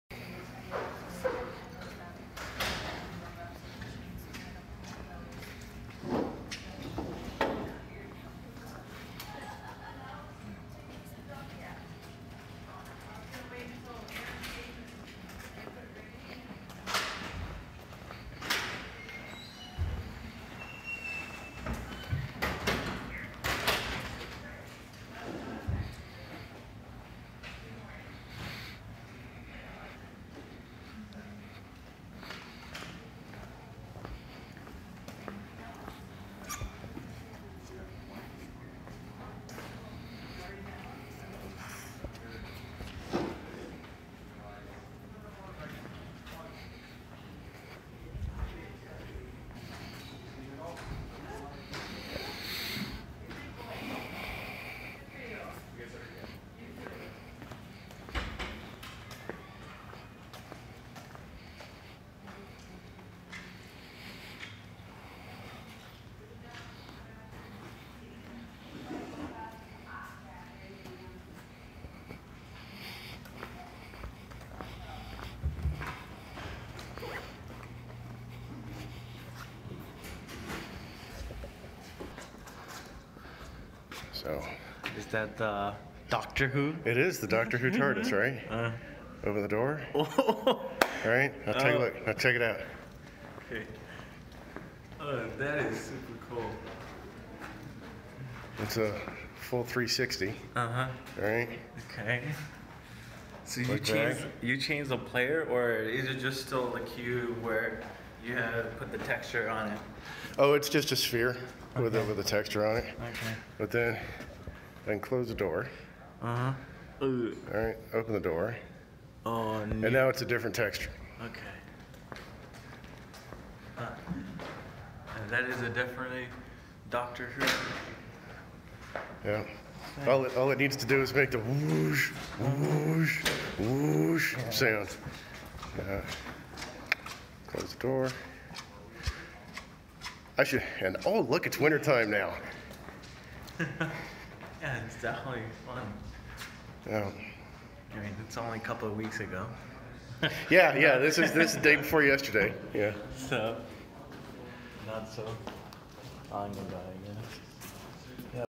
Tardis 360
An AR Experiment done with Google's AR Core, and Unity 2017.   The TARDIS is placed on the ground plane, and like any good TARDIS, it's bigger on the inside.